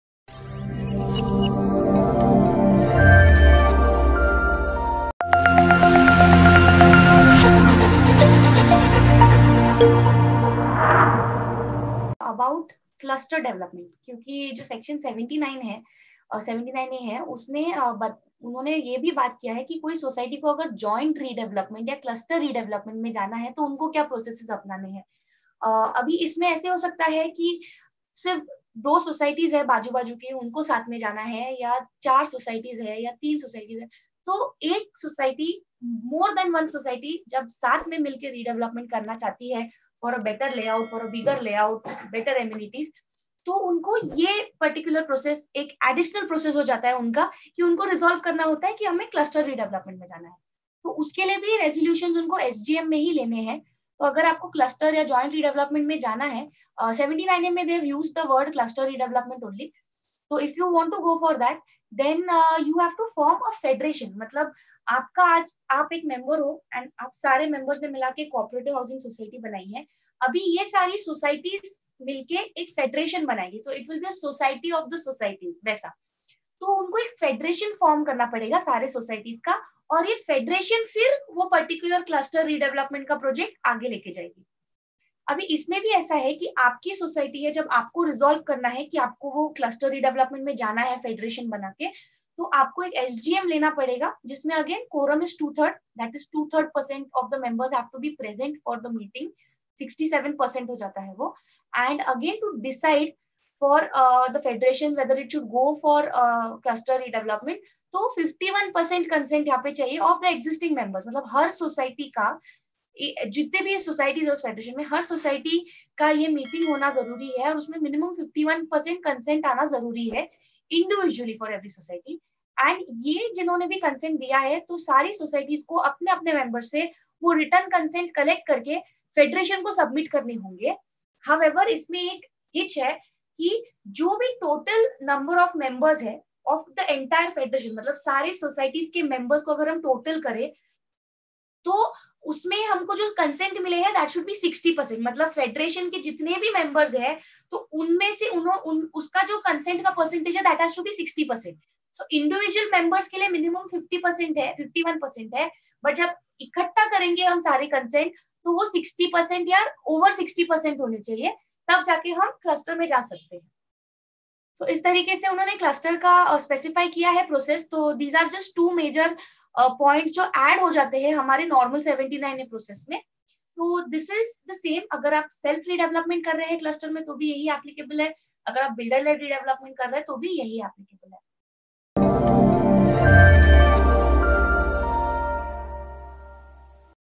What is the process for Joint Redevelopment or Cluster Redevelopment?
Mrs. Nupur Gaitonde, CA,Director, Toughcons Swanirman, provides clarity on Section 79A. Issue of Notice,Meeting Quorum & Minutes,Consent from Members,PMC Selection,Selection of Developer/ Contractor,Cluster Development.